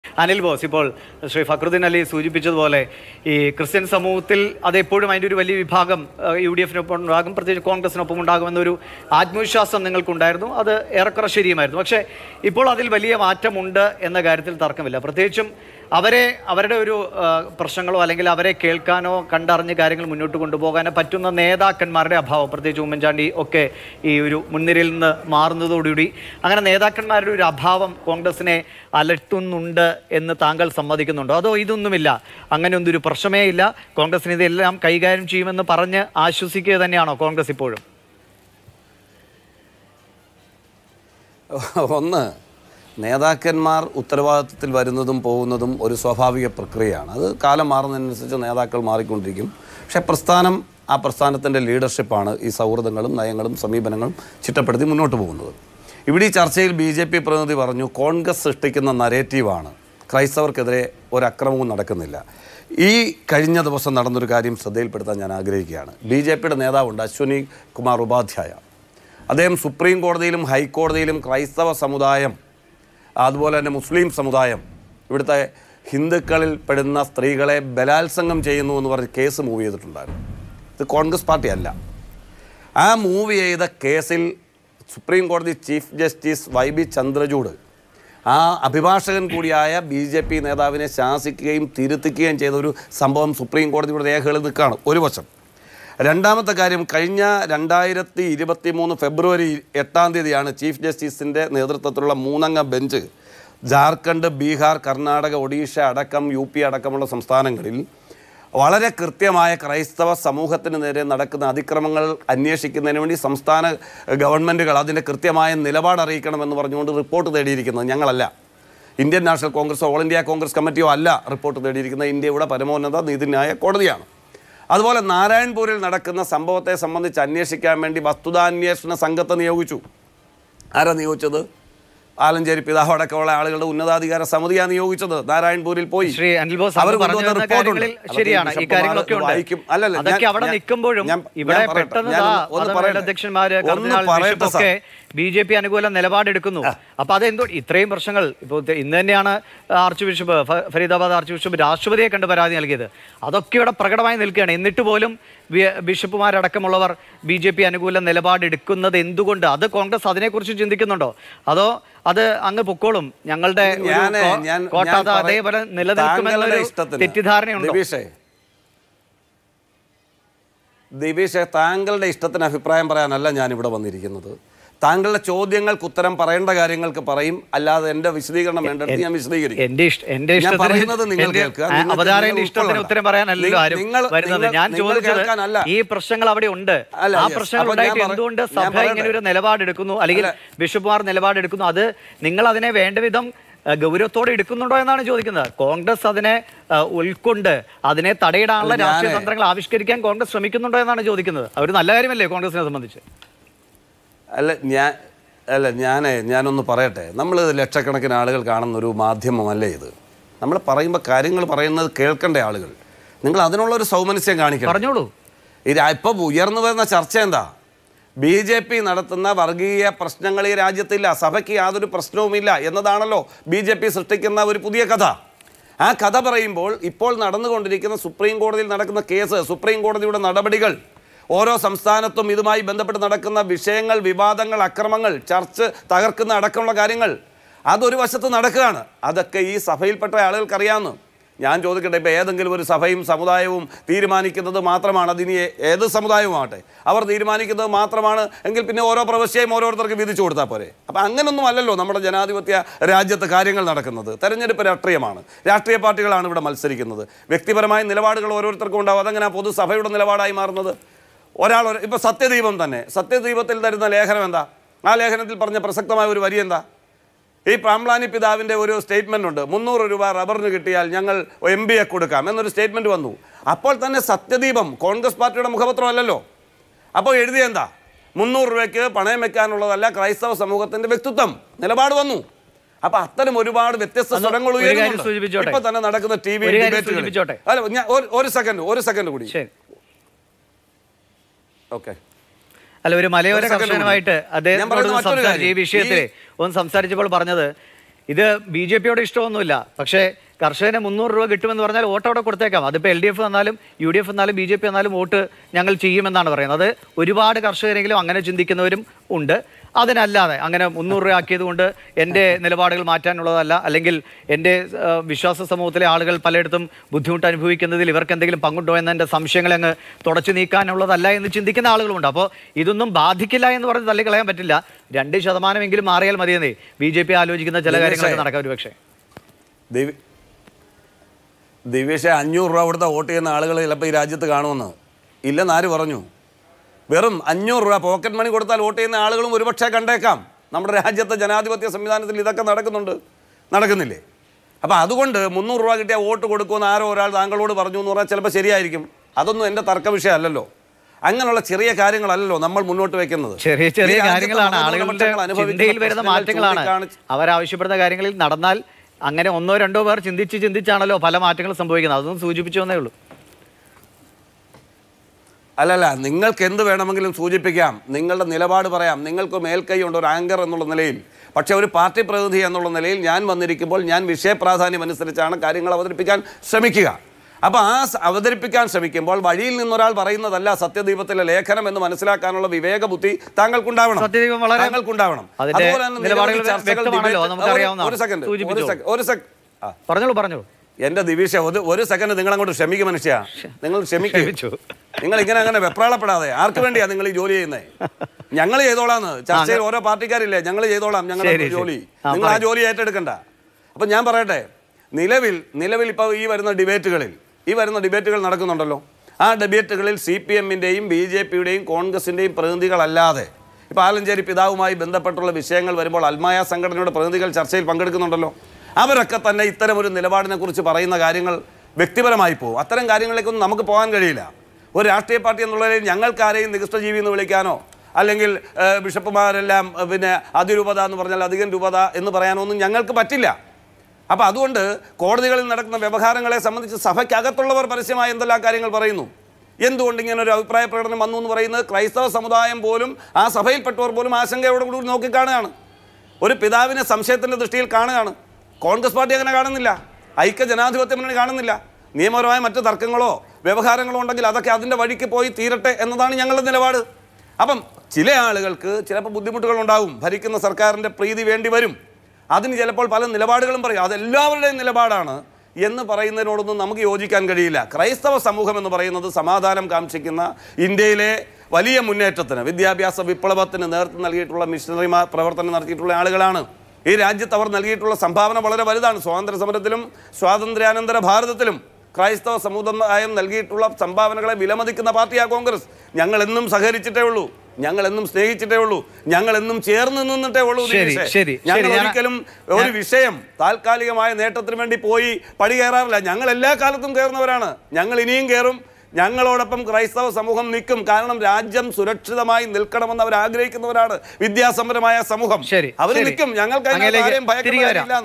'ഏതെങ്കിലും സഭ തീരുമാനിക്കുന്നത് പോലെയല്ലല്ലോ തിരഞ്ഞെടുപ്പില്‍ സംഭവിക്കുന്നത്'| Mathrubhumi News
വ്യക്തിപരമായ നിലപാടുകള്‍ ഓരോരുത്തര്‍ക്കും ഉണ്ടാകും. അത് എങ്ങനെയാണ് സഭയുടെ നിലപാടായി മാറുക എന്ന് അനില്‍ ബോസ്‌

Happy viewing!